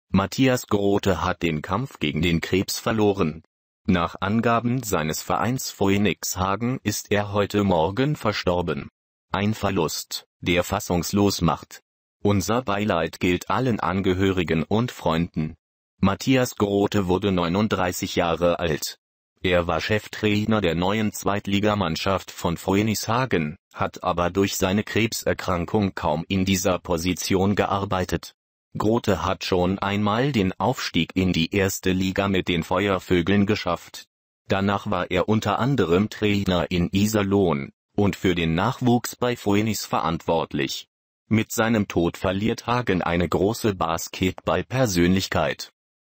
Matthias Grothe hat den Kampf gegen den Krebs verloren.
Matthias Grothe hat den Kampf gegen den Krebs verloren. Nach Angaben seines Vereins Phoenix Hagen ist er heute Morgen verstorben. Ein Verlust, der fassungslos macht. Unser Beileid gilt allen Angehörigen und Freunden. Matthias Grothe wurde 39 Jahre alt. Er war Cheftrainer der neuen Zweitligamannschaft von Phoenix Hagen, hat aber durch seine Krebserkrankung kaum in dieser Position gearbeitet. Grothe hat schon einmal den Aufstieg in die erste Liga mit den Feuervögeln geschafft. Danach war er unter anderem Trainer in Iserlohn und für den Nachwuchs bei Phoenix verantwortlich. Mit seinem Tod verliert Hagen eine große Basketballpersönlichkeit.